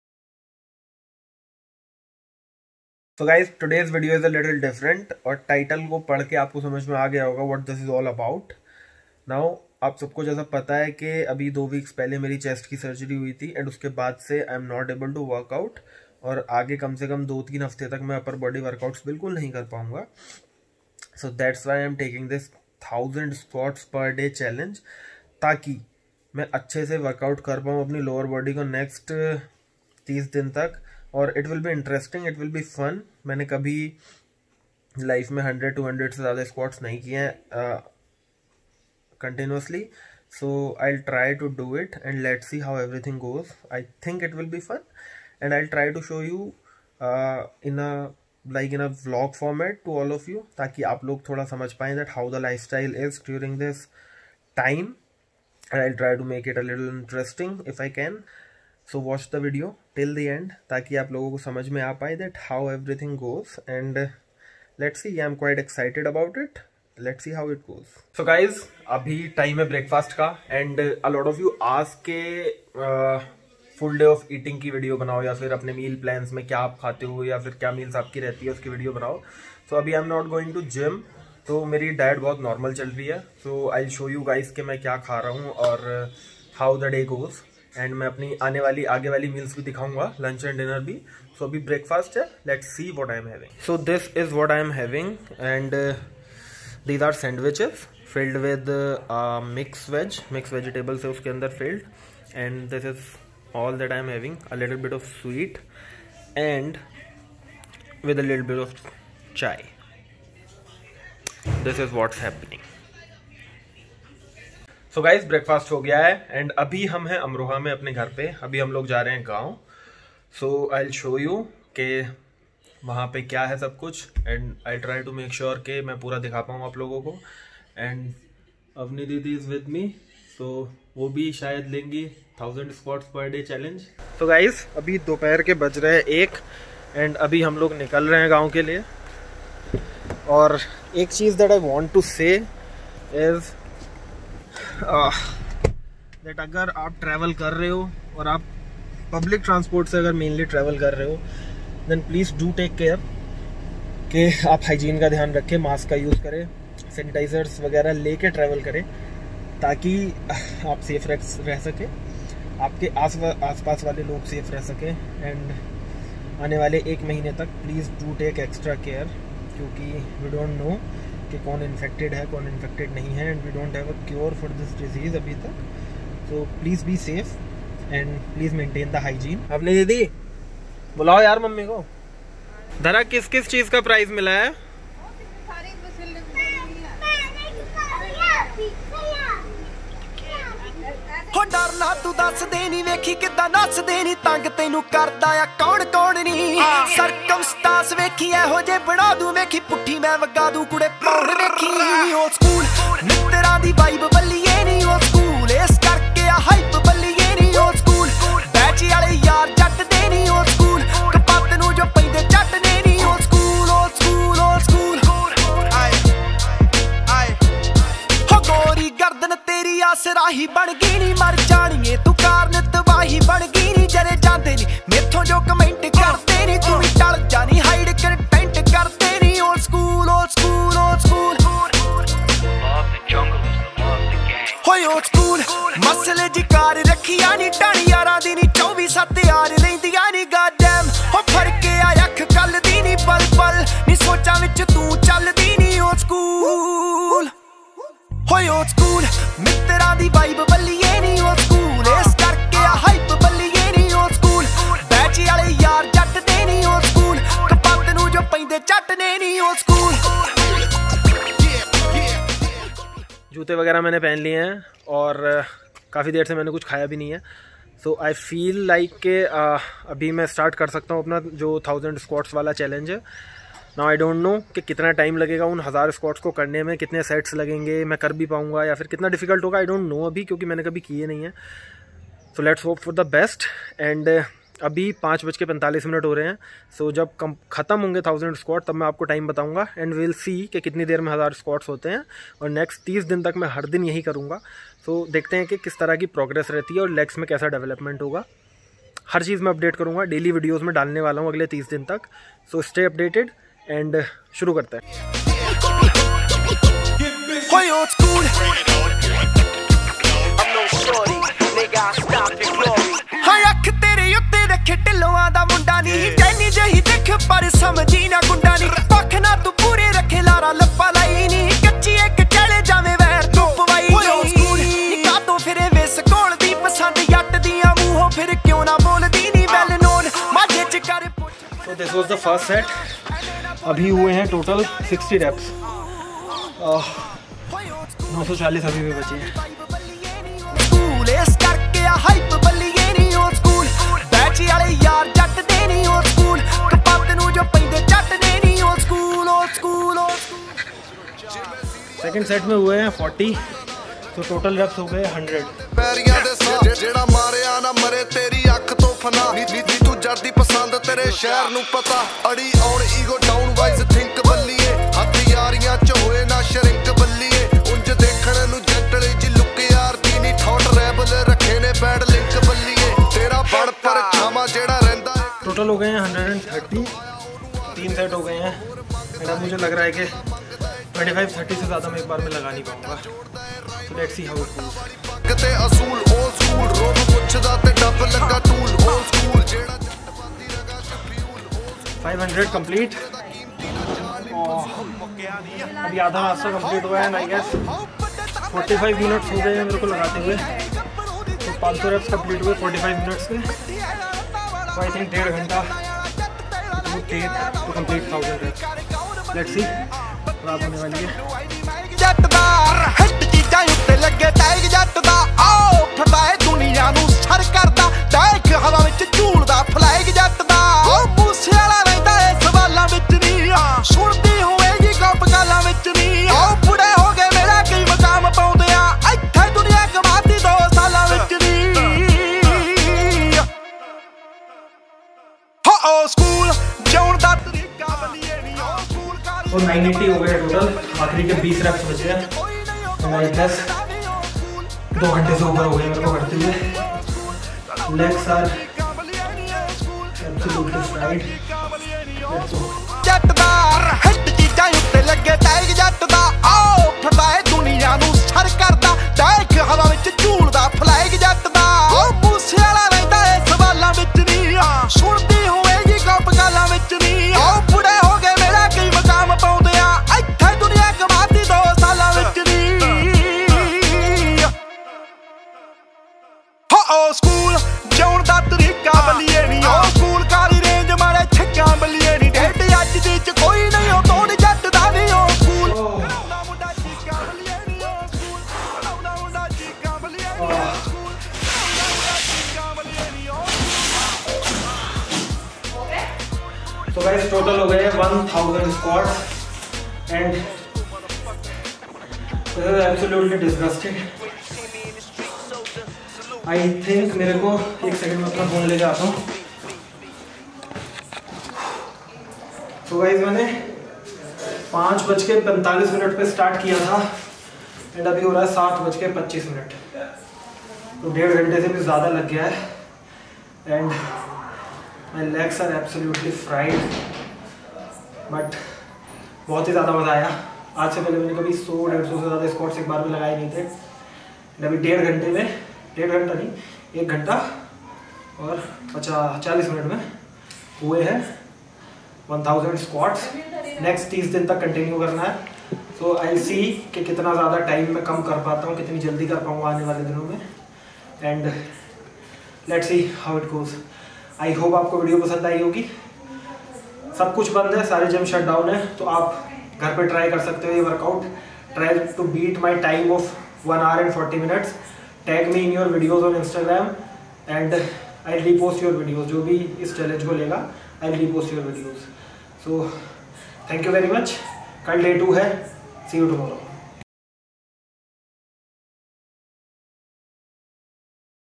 Day 1 Of 1000 SQUATS Everyday For 30 Days
I am gonna do 1000 squats every day for the next 30 days. Let's see how it goes.